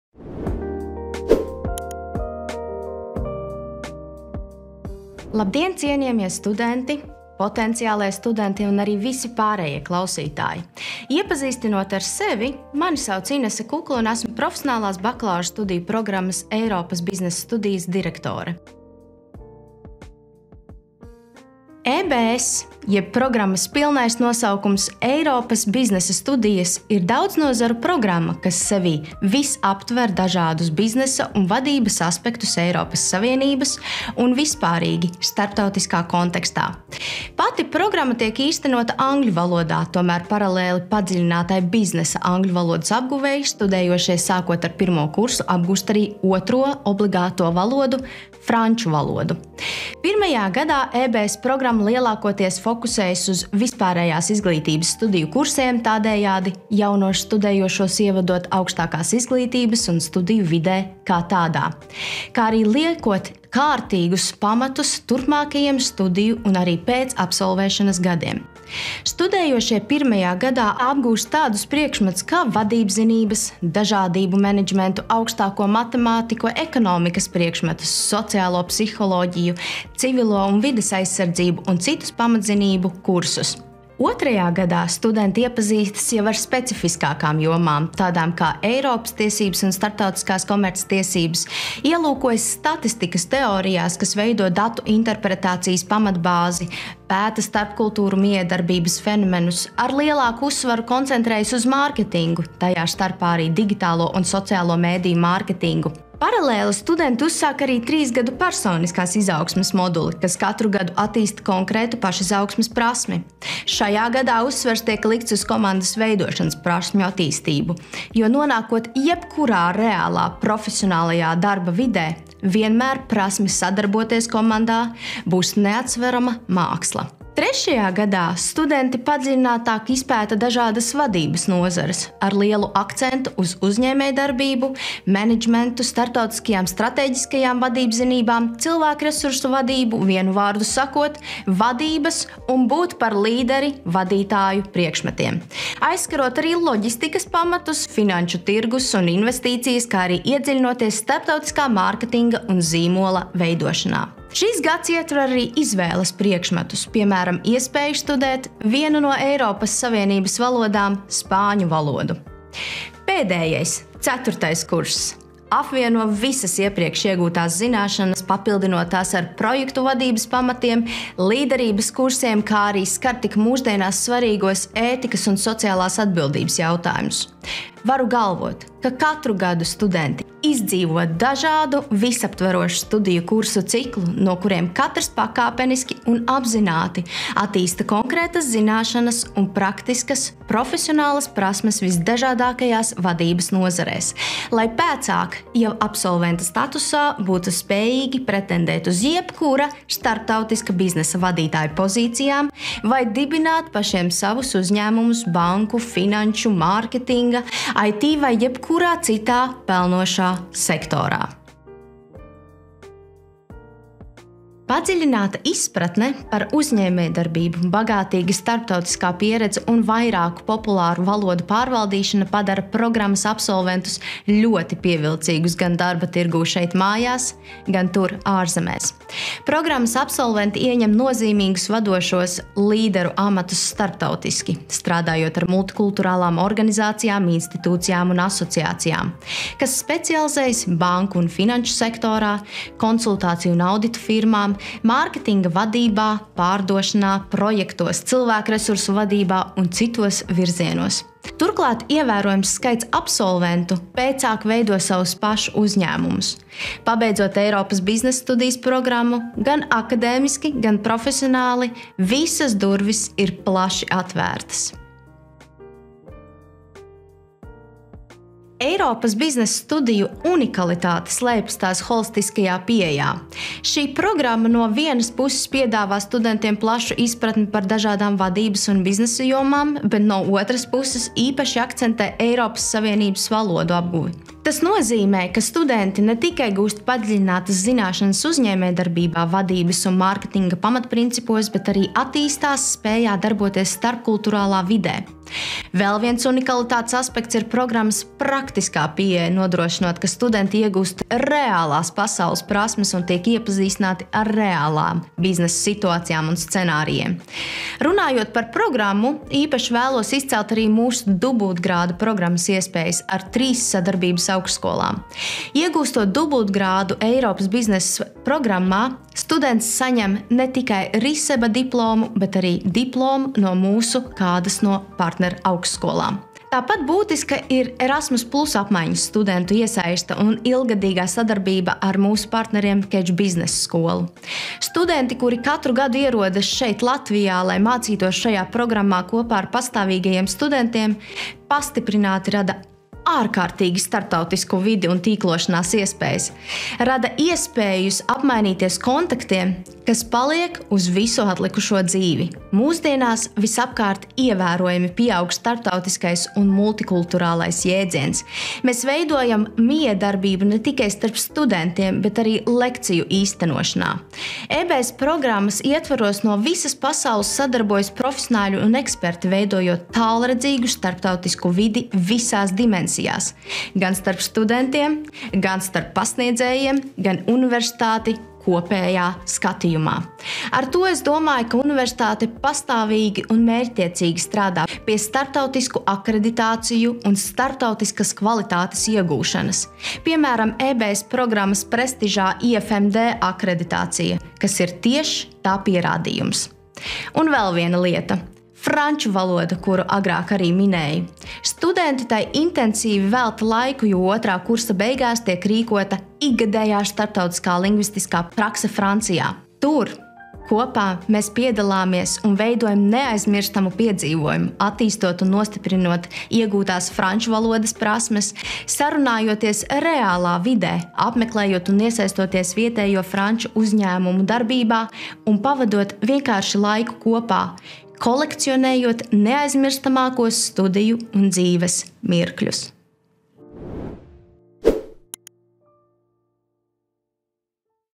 Bakalaura studiju programma "Eiropas biznesa studijas" augstskolā RISEBA
Atklāj savu globālās līderības potenciālu! 🌐

Sāc studēt "Eiropas biznesa studijas" programmu, kas izstrādāta, lai veidotu nākotnes vadītājus un nozares līderus multikulturālai, globālai dzīvei.
🚀 Iepazīstini sevi ar jaunākajā, tendencēm, kas virza starptautisko uzņēmējdarbību, veicinot vidi, kas piesaista studentus  un pasniedzējus no visas pasaules.

👉 Esi globālās uzņēmējdarbības līderu priekšgalā un uzzini vairāk šeit: riseba.lv/programma/eiropas-biznesa-studijas/